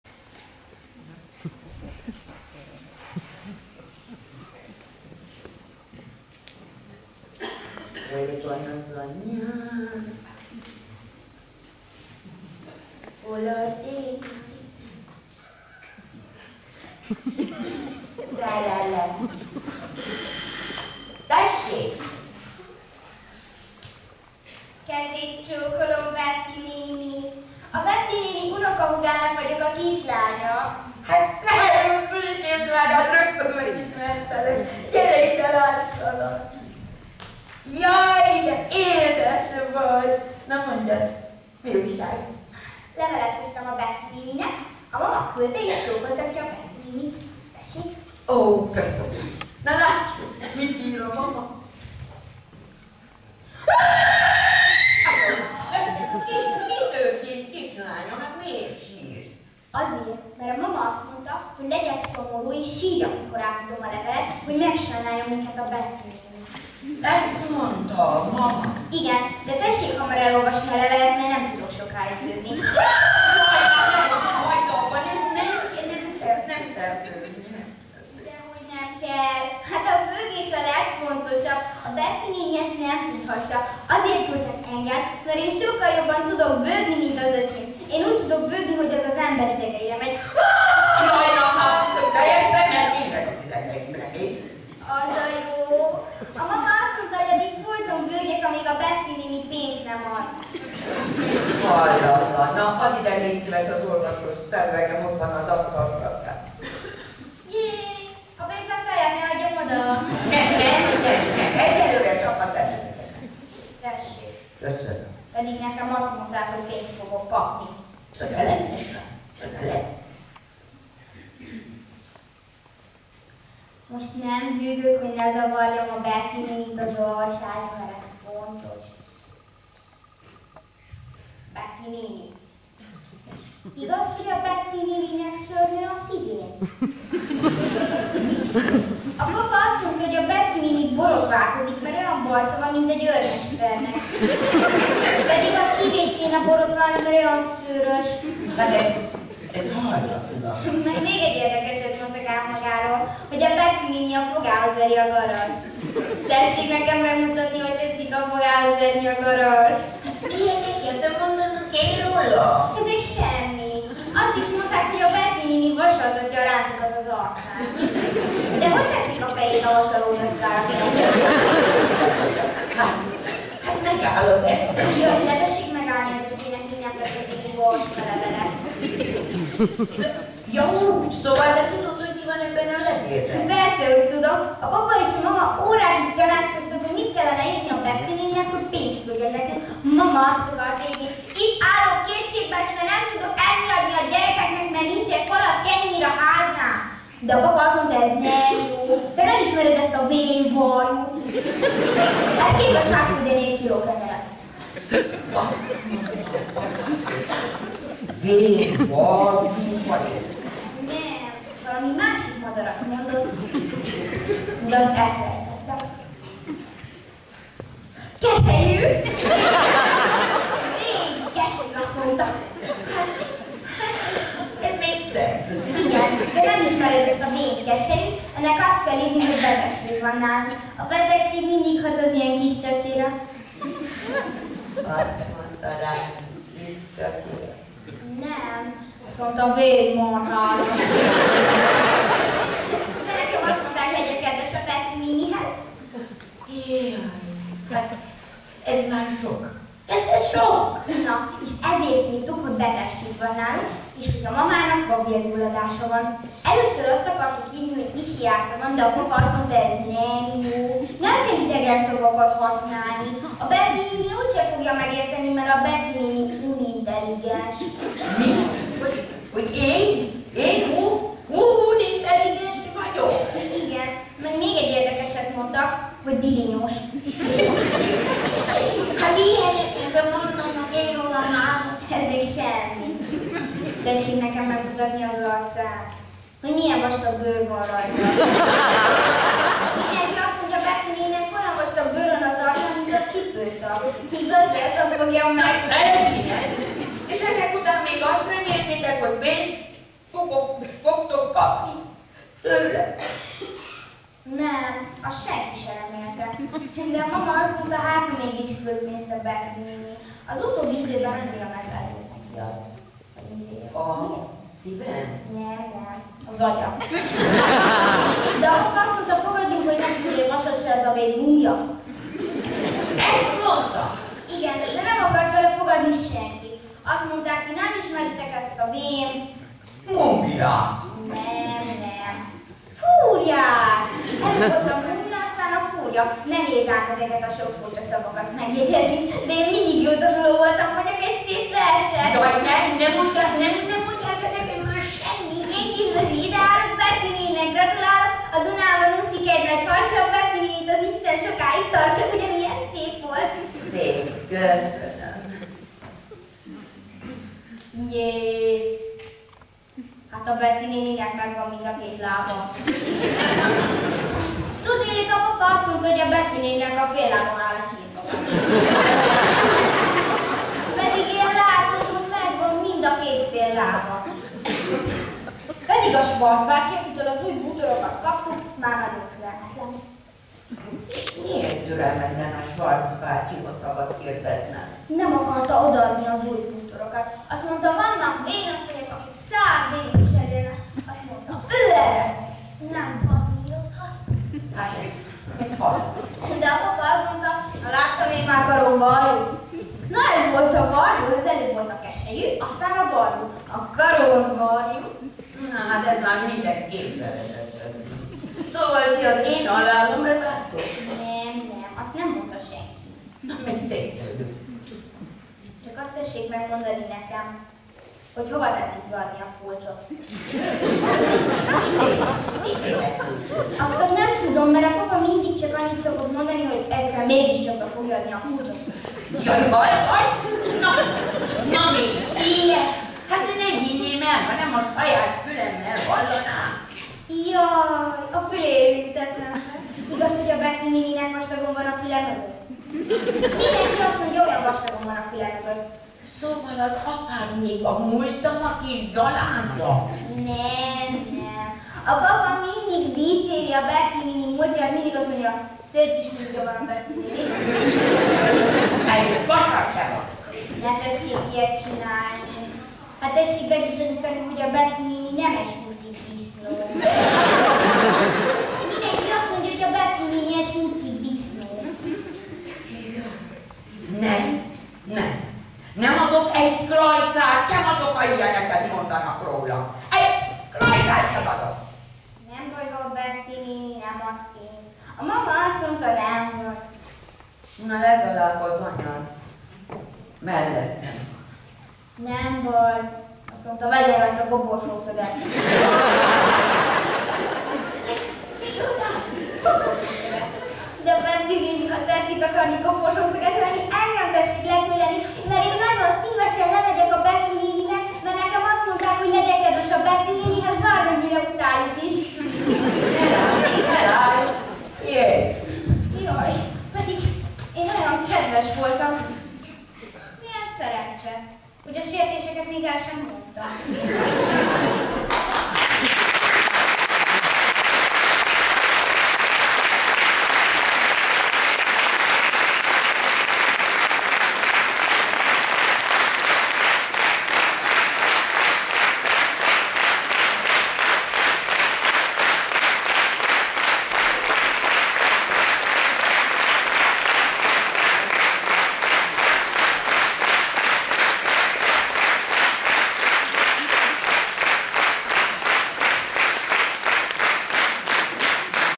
Vaszary Gábor: Bözsi mindent elintéz
Elhangzott a Horváth János Társaság színjátszó csoportjának 2020-as kabaréműsorán. Szereplők: Szegedi-Kiss Gyöngyi és Szabó Dorottya.